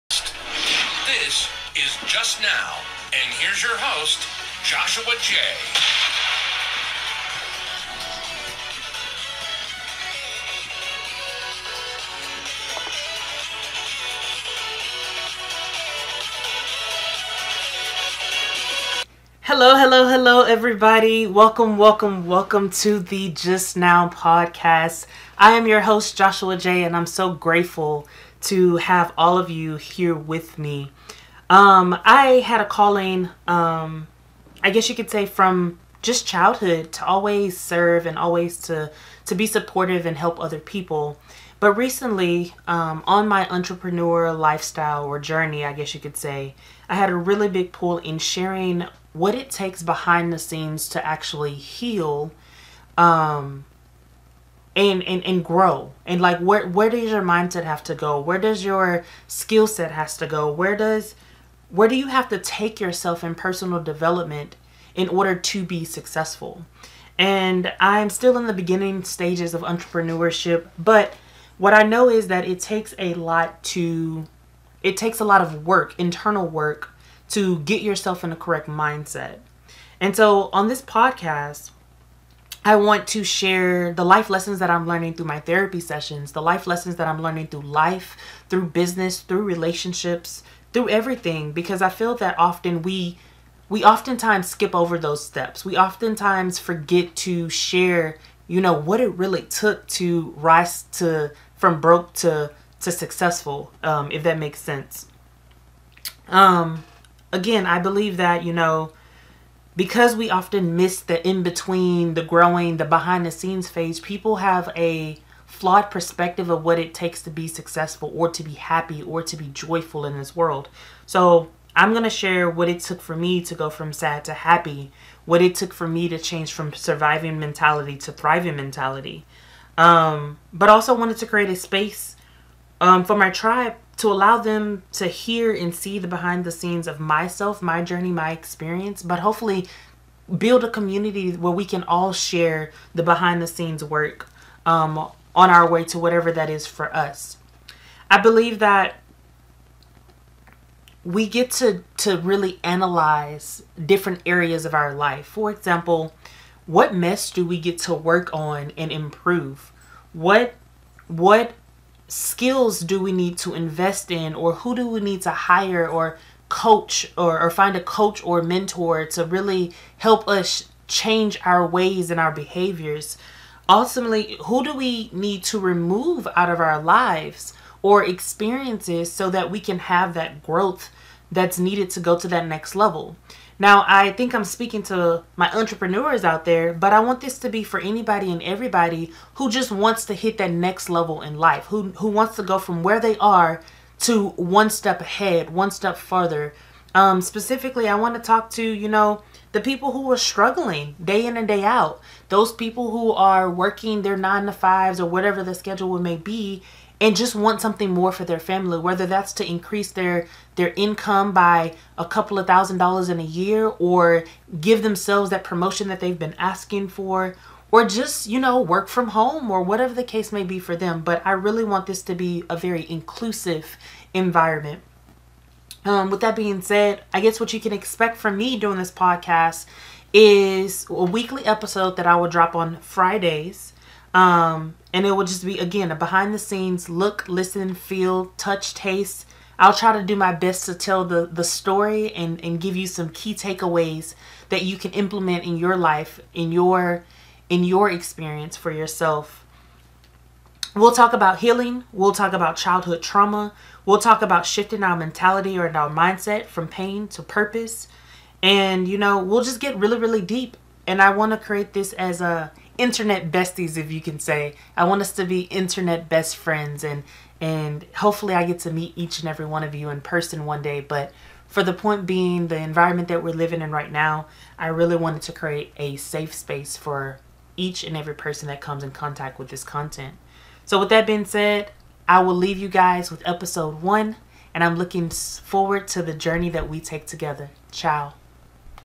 Podcast intro
Welcome to the podcast Just Now!!! This episode is a welcome to the tribe and community and why i created this podcast. This podcast is for entrepreneurs who want support in healing traumatic experiences in their life so they can grow their wealth. This podcast is for the persona who wants to heal and grow and achieve their next level.

I wil give you real life examples, tools and resources to use in your everyday life to heal your pain and turn it into passion! We will get real, raw and deep on this podcast and become internet besties :)

I am dedicated to healing the world through LOVE and LIGHT, so peace, love and happiness to you all!!